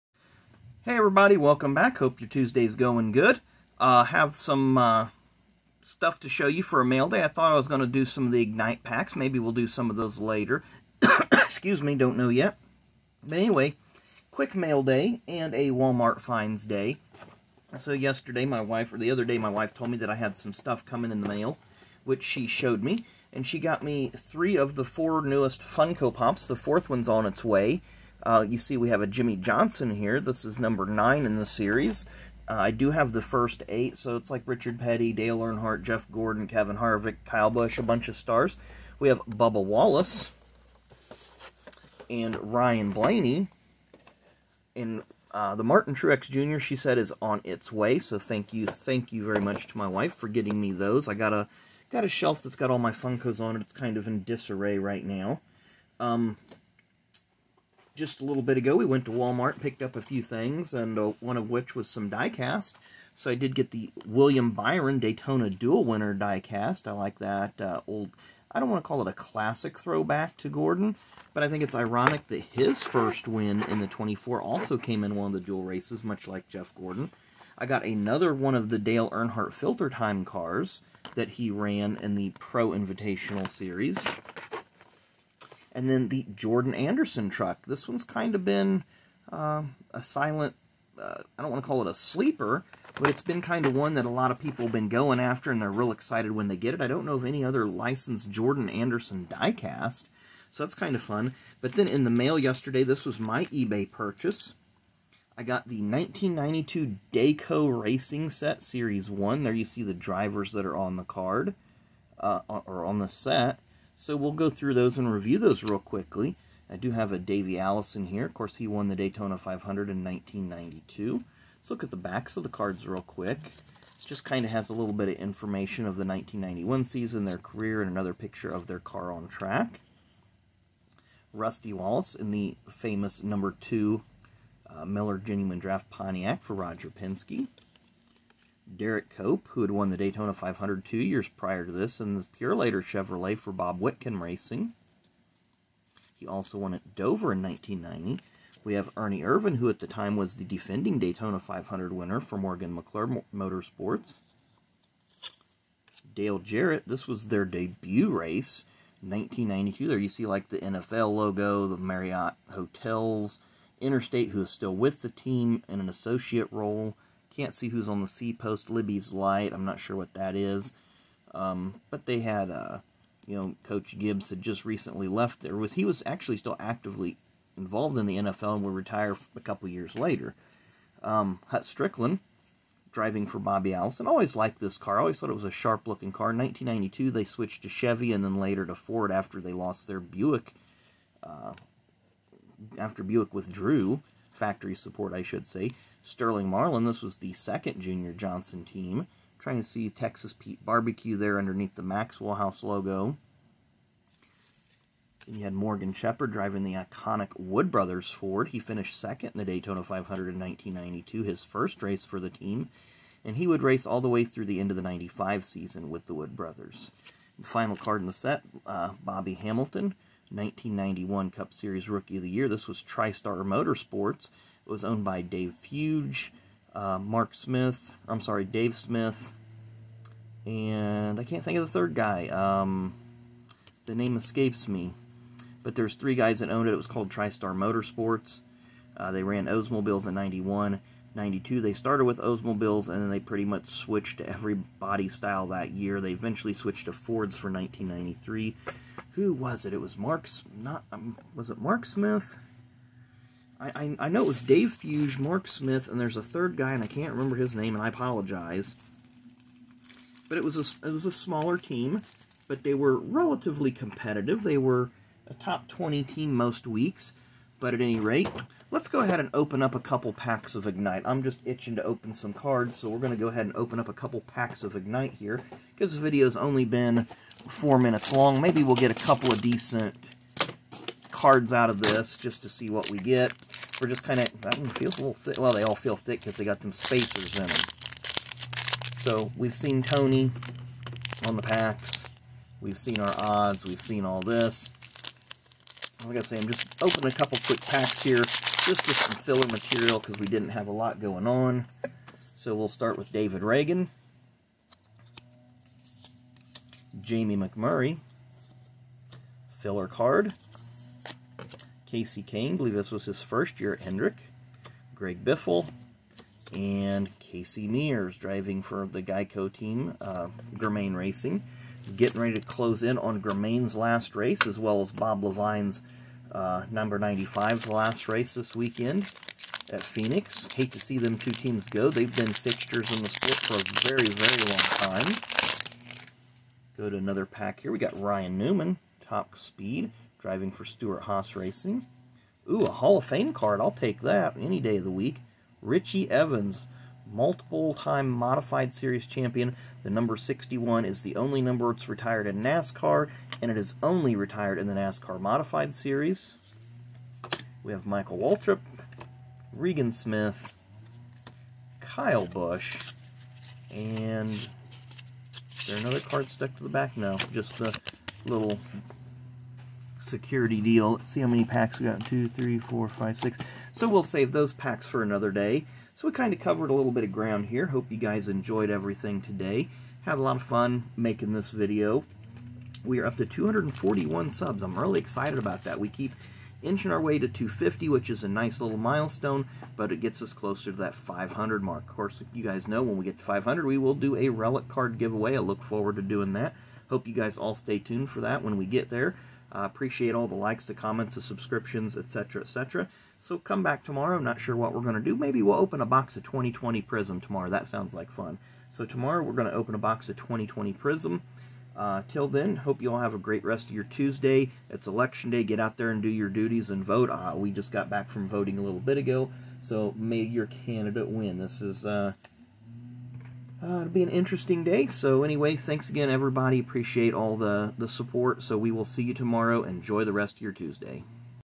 Mail day Wal Mart finds Nascar die cast 1992 Dayco cards & Funko Pops plus two pack rip 2012 Ignite
A mixture of cards and diecast plus a bonus rip.